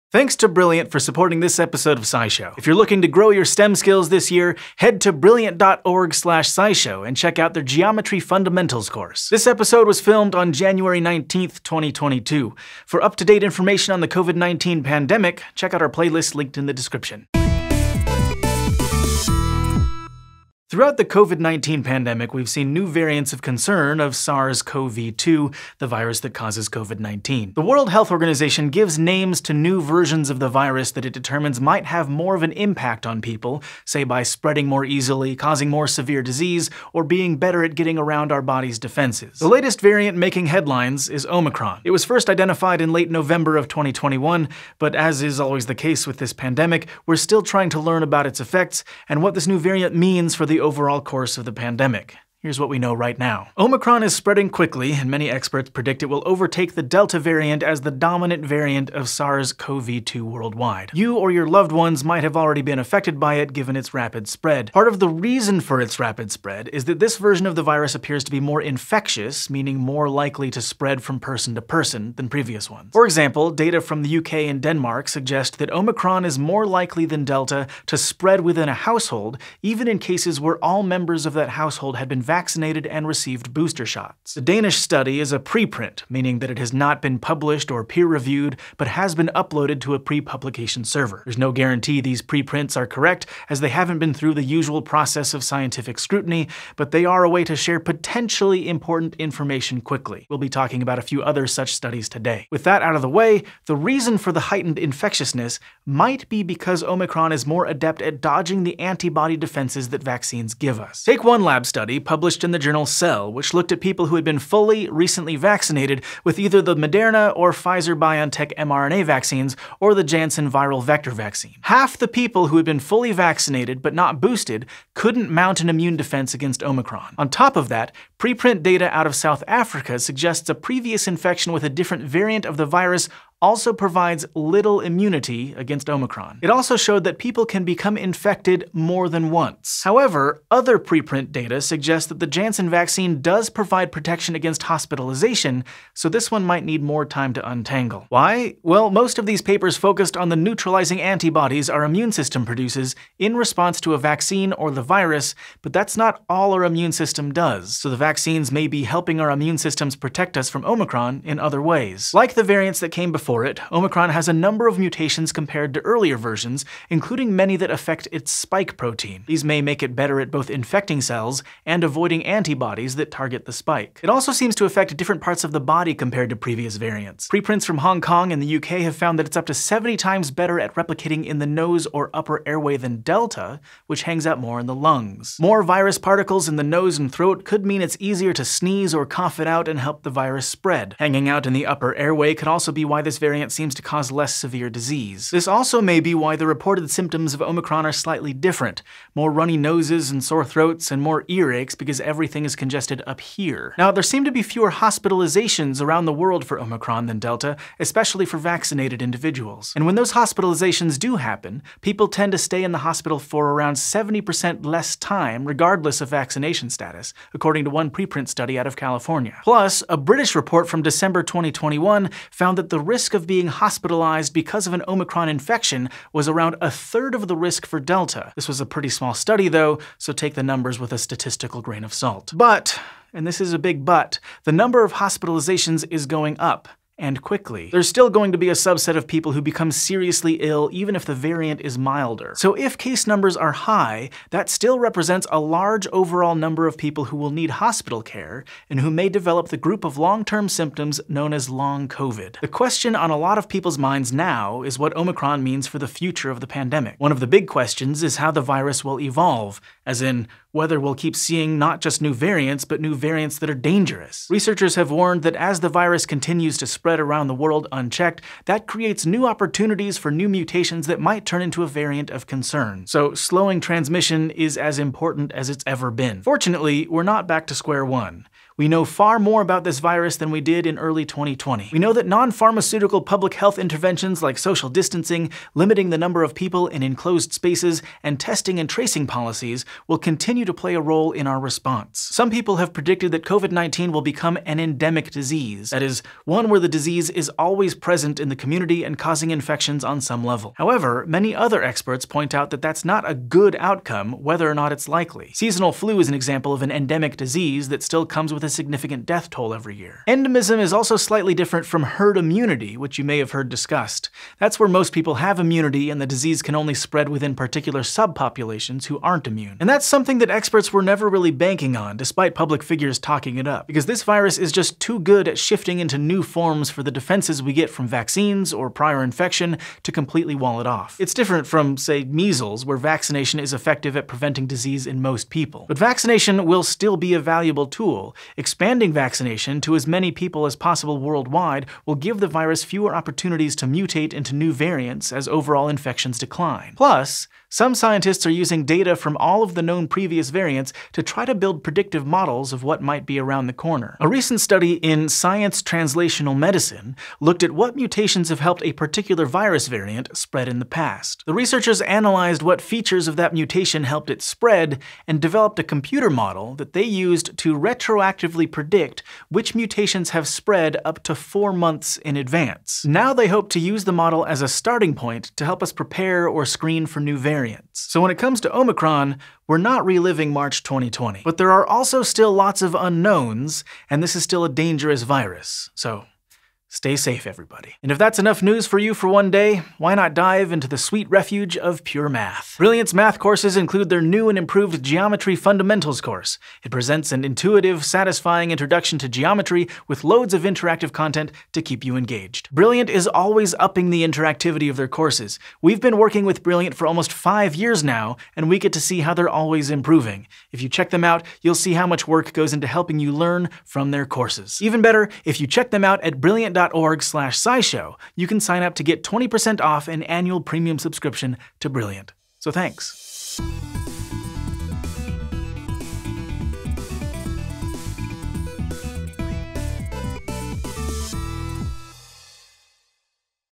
What Omicron Means for the Pandemic’s Future | SciShow News
New variants of SARS-CoV-2 have emerged throughout the COVID-19 pandemic, with the latest one being Omicron. We’re still trying to learn about its effects and what it means for the overall course of the pandemic, but here’s what we know right now.